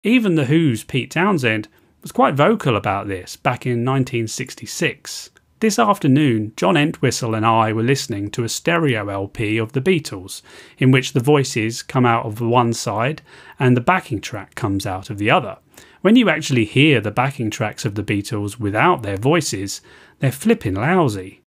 What did Pete Townshend think of The Beatles?
Pete Townshend drops truth bombs on The Beatles' mixes! 💣 Would the new remixes impress him, or do they fall short even for rock legends?